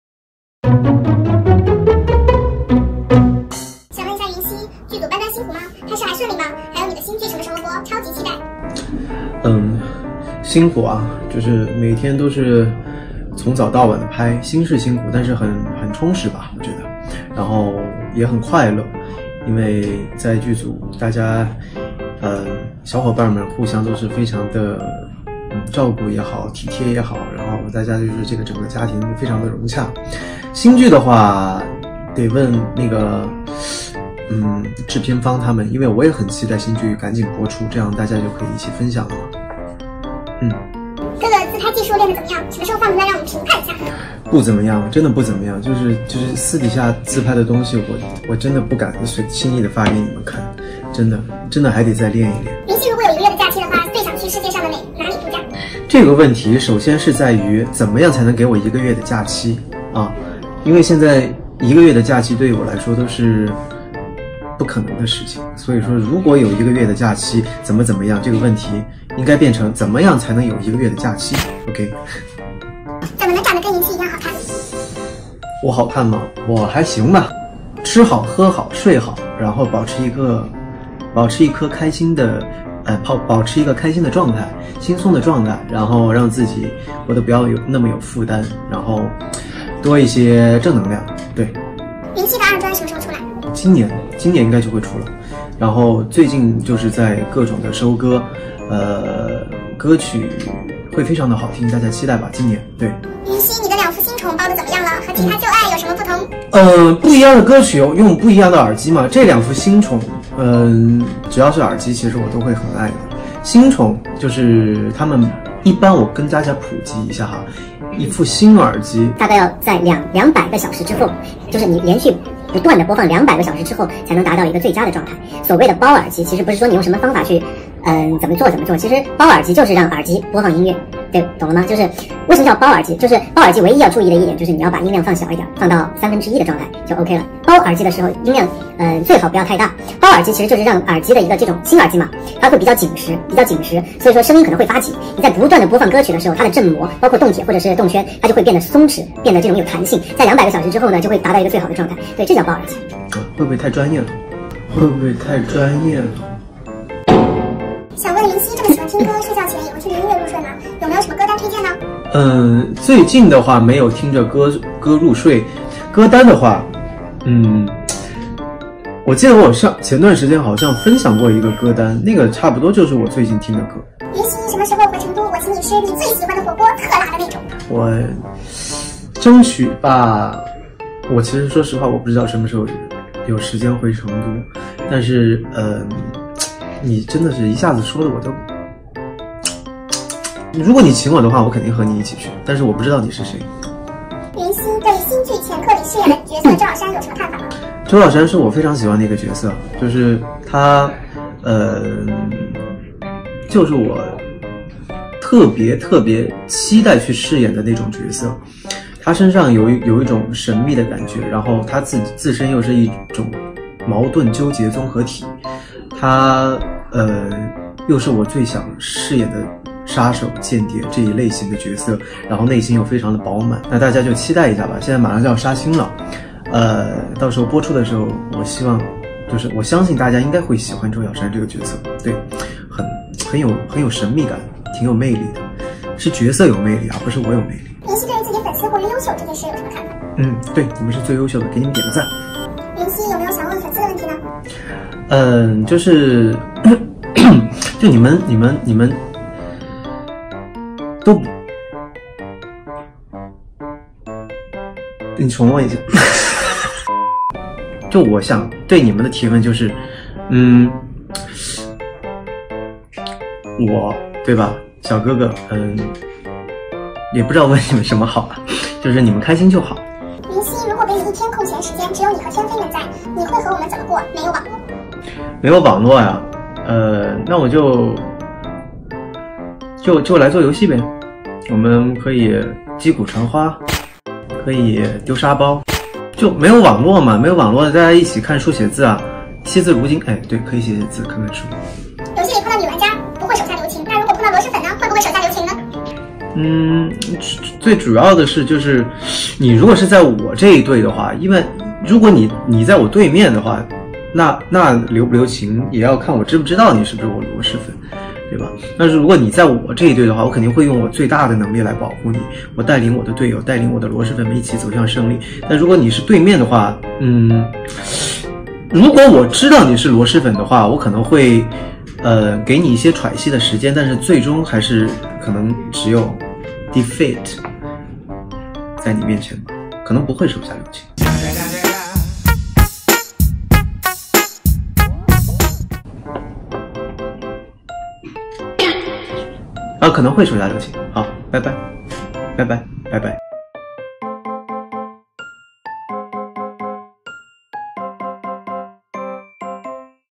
[ENG SUB] 03-23-19 You Ask, and Leo Will Answer 你问云熙来答 【罗云熙 | Yunxi Luo | Runyu | 라운희 | 윤옥】
Leo answered frequently asked fan questions in his own quirky fashion! What made him super talkative? Why did things turn awkward all of a sudden? Why was he uttering something extreme? You'll find out! Seriously Bro, what were you trying to ask us?
云熙用他一如既往清奇的方式来回答罗狮粉们的问题? 他为何开启话唠模式? 为何空气突然安静? 又是什么问题让他口出狂言? 进来看看吧！敢问小哥哥，你到底想问我们什么呀?

From: 罗云熙工作室
Translation：企鹅
Proofreading：企鹅/Phoebe Wu
Subtitle：心肝
Abstract：企鹅